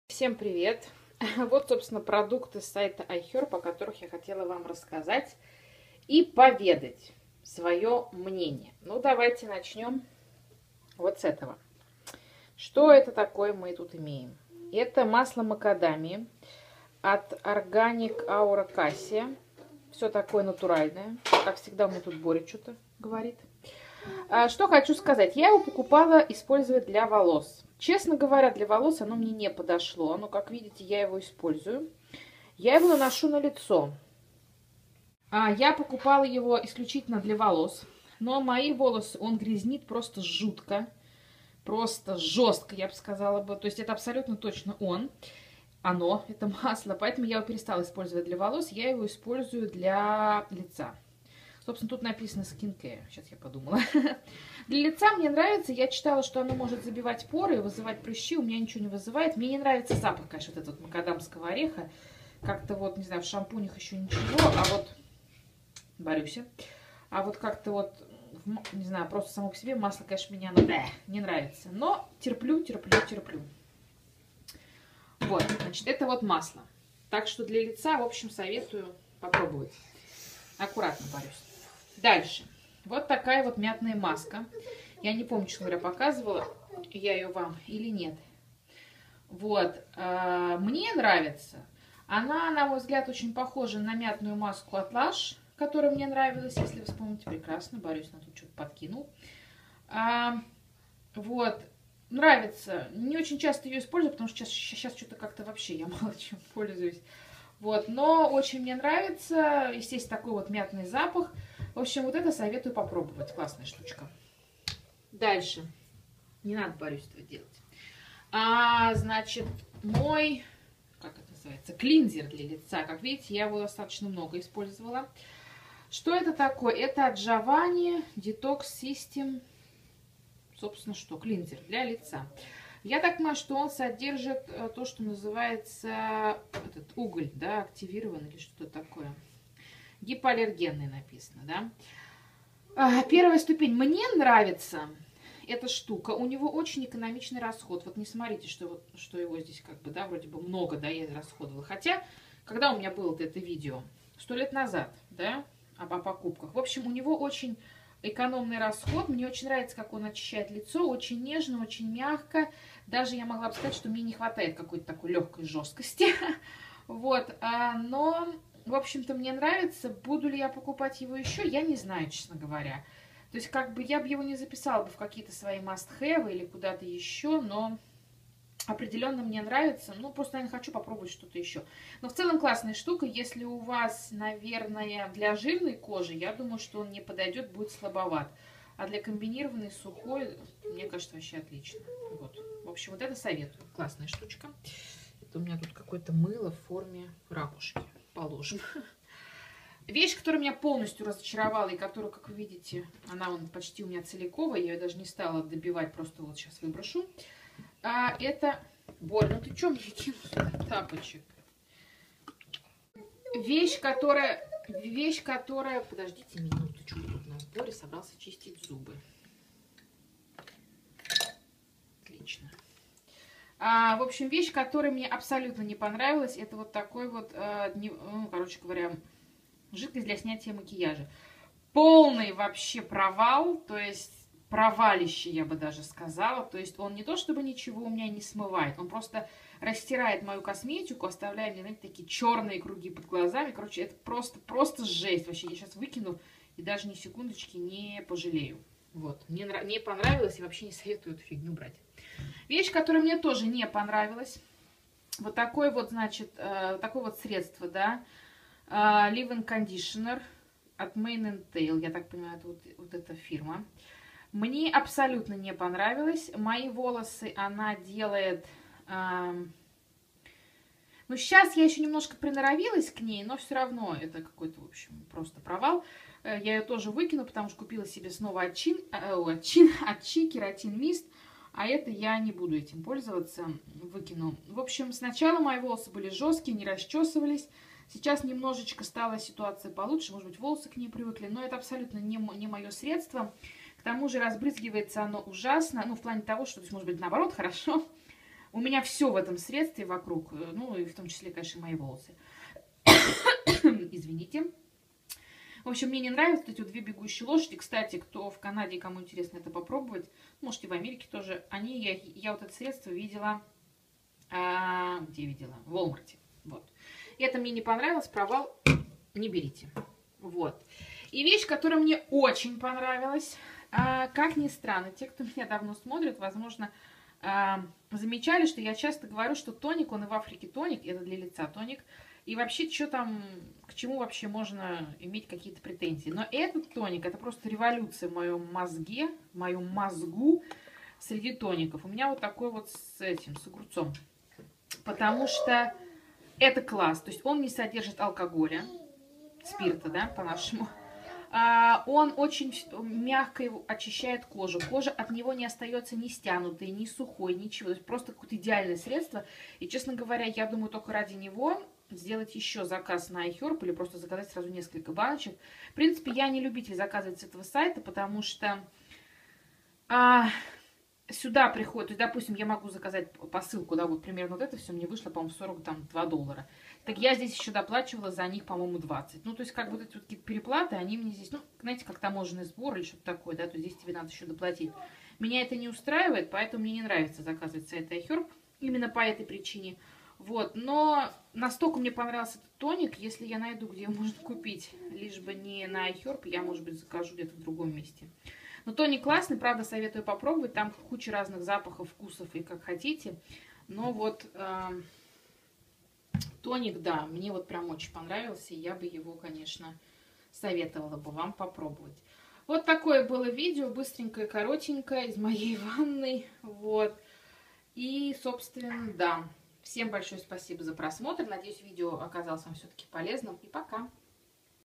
Отзыв продукты с iherb, уход за лицом: тоники, маски, масло макадами
Отзыв продукты с iherb, уход за лицом: тоники, маски, масло макадами Масло макадами. Маска для лица с ментолом. Умывалка для лица . Жидкость для снятия макияжа. Средство для волос. Тоник для лица с огурцом.